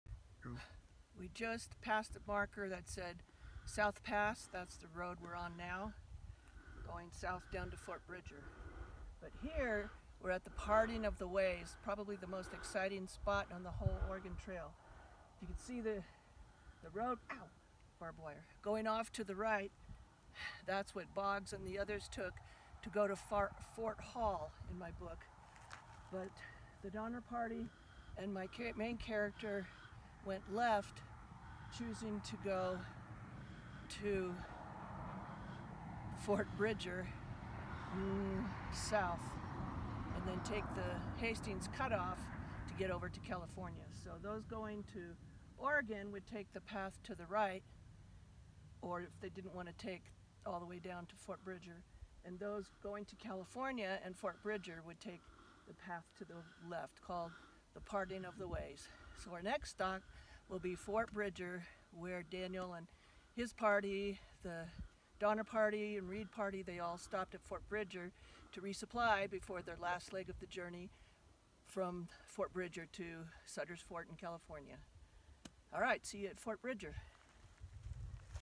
Parting of the Ways on the Oregon Trail
In "California Trail Discovered," the characters in my historical fiction book took the trail to the left to head toward Ft. Bridger instead of taking the right-handed trail toward Ft. Hall at this "Parting of the Ways" along the Oregon Trail. Installment #8